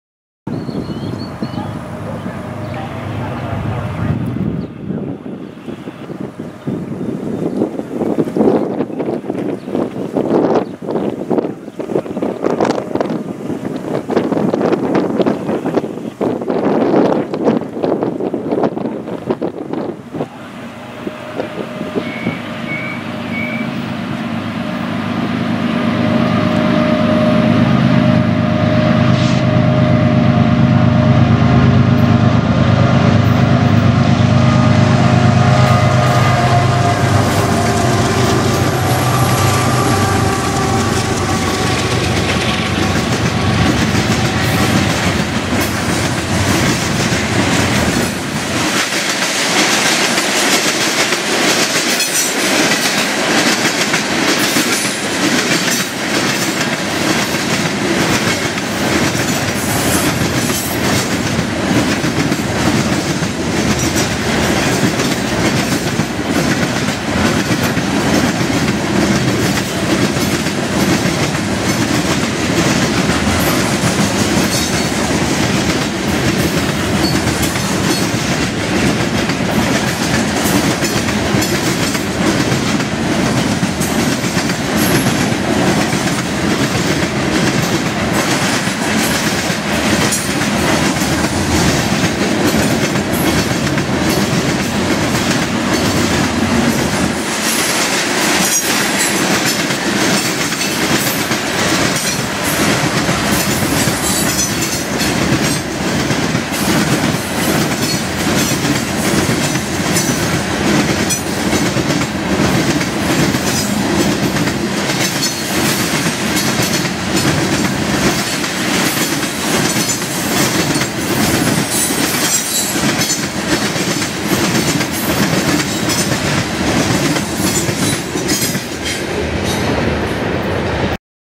cml760 11 UP L Rock 24 Apr 21
cml760 – UP L. Rock train 24 Apr 21 – Martinez Park:  Martinez Park is at the intersection of Saltillo Street and Trinity Street next to the park of the same name.  This UP eastbound L. MWAX Rock train with 100 cars is on the Del Rio Sub #1 main.  I’m pretty sure I’ve mentioned before that Martinez Park is a good place on the west side of town to get both eastbound and westbound trains; however, there is no shade, so we tend to avoid it in the very hot South Texas Summers!  Carl Lehman